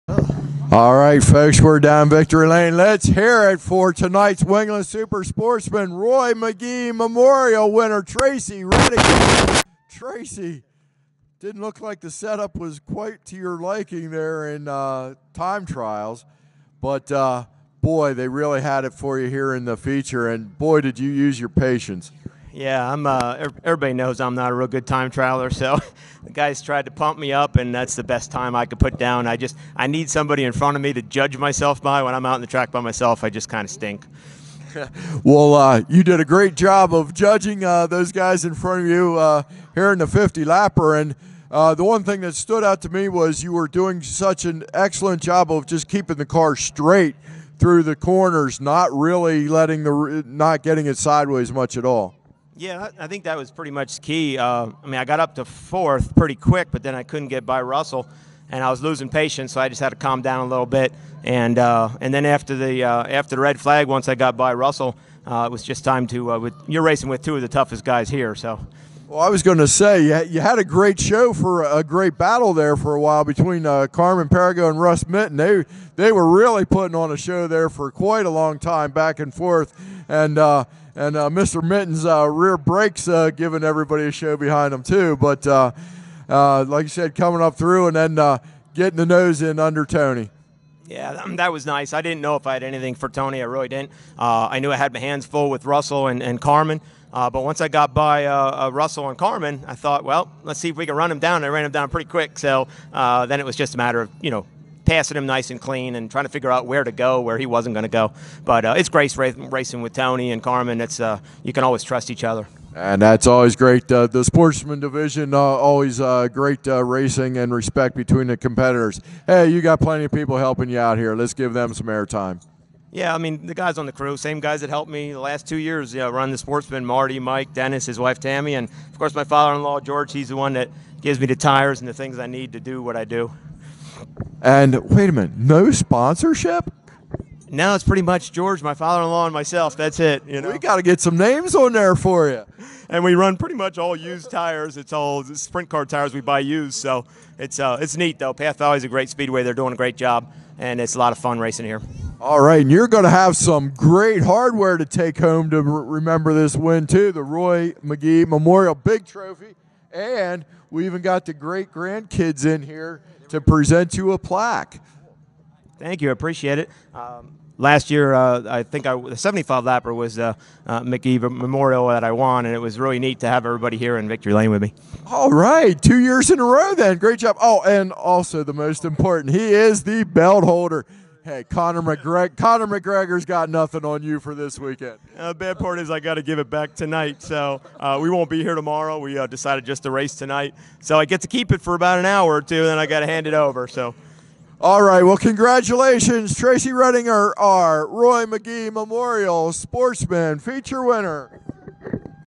Tracy Readinger in victory lane at Path Valley 8-25-17
Tracy Readinger in victory lane after winning the Roy McGee Memorial at Path Valley Speedway.